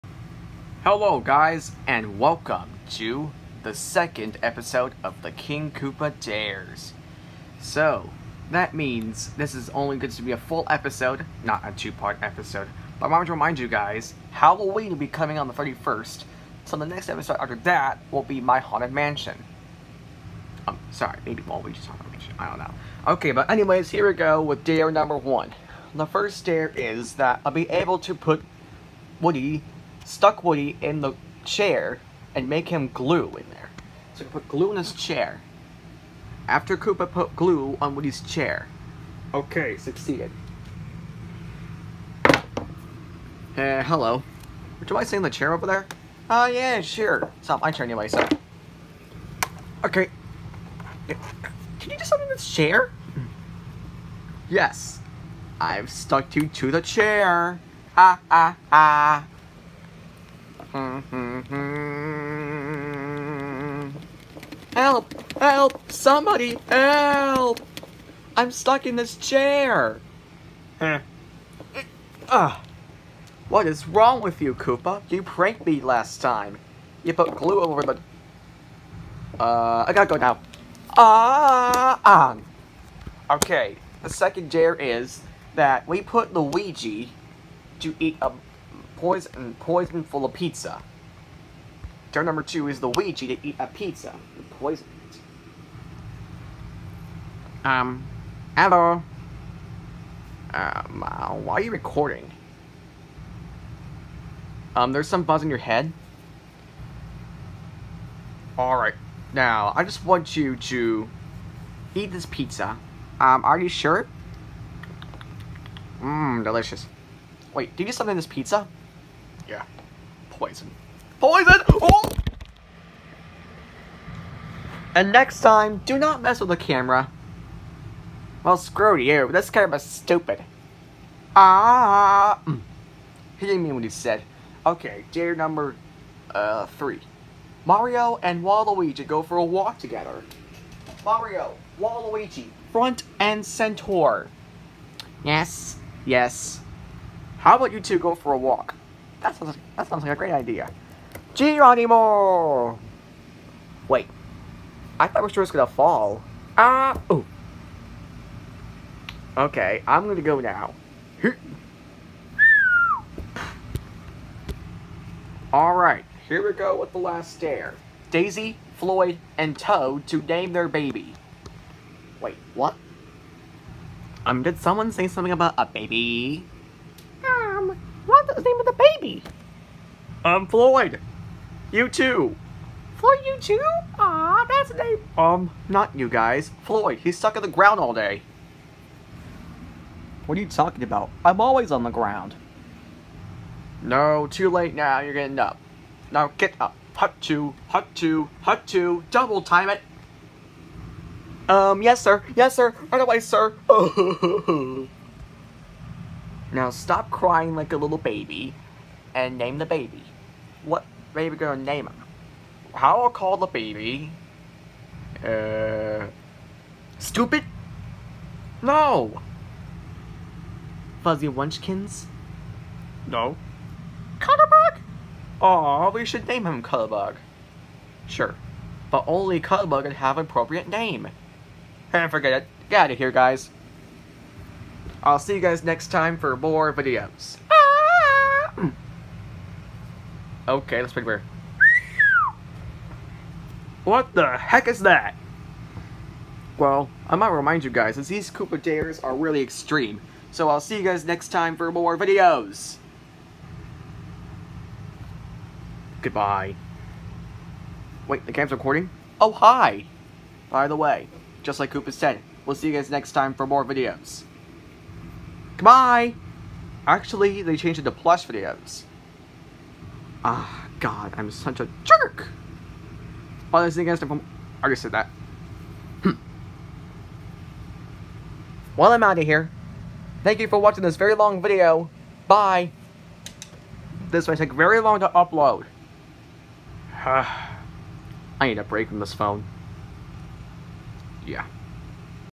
A New Plush Paradise Season 2 Episode 5: King Koopa’s Dares 2
This is probably the longest video of my channel. So these 4 dares are the dares of today.

Chapters:

0:00 Chapter 1: Introduction

0:16 Chapter 2: The Dares Begins!

0:49 Chapter 3: More Dares

1:30 Chapter 4: More More Dares

2:00 Chapter 5: Why Not More Dares?

2:50 Chapter 6: Daisy, Floyd, and Toad Naming Their Baby

3:50 Chapter 7: Ending

4:48 Chapter 8: Sign Off